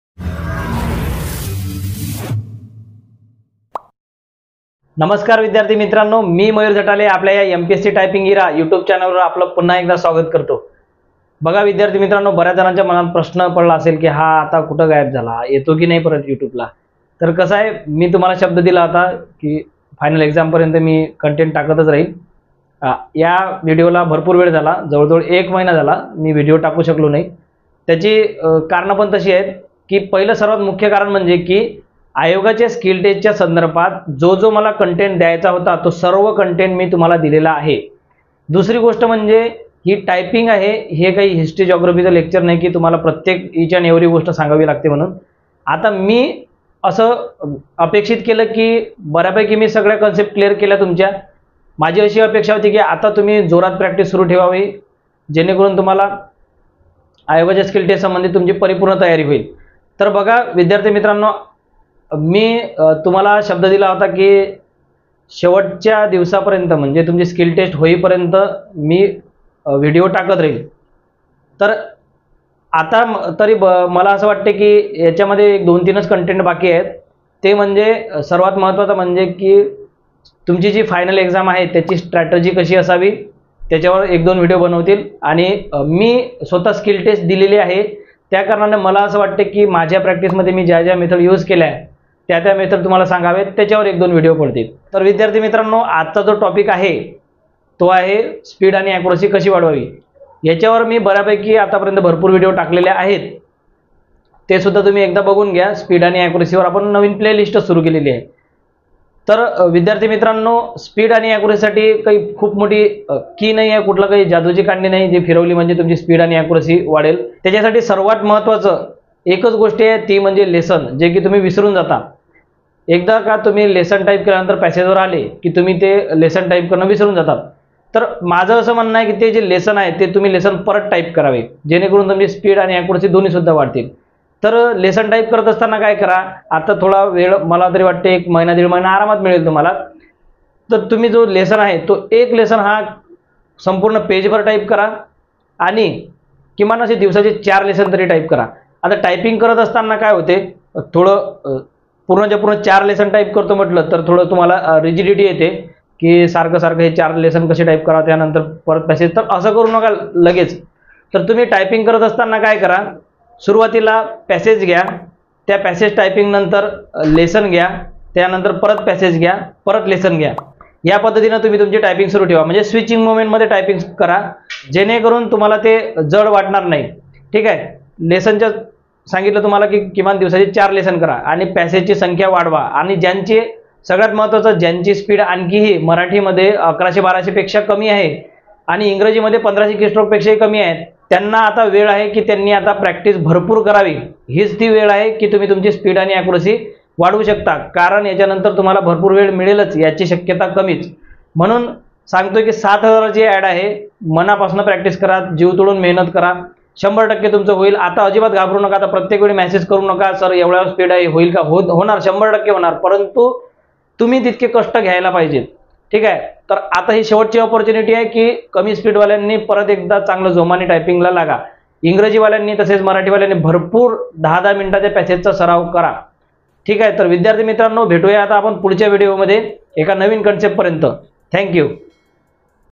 How To Increase Speed And Accuracy || Mpsc Typing Era
नमस्कार विद्यार्थी मित्रांनो आणि मैत्रिणींनो, Mpsc Typing Era या YouTube channel वर सर्वांचे मनापासून स्वागत आहे. या video मध्ये लिपिक टंकलेखक (७०००+) तसेच टॅक्स असिस्टंट साठी होणाऱ्या MPSC Skill Test विषयी मार्गदर्शन करण्यात येत आहे.

  Telegram Channel ला जॉईन करा.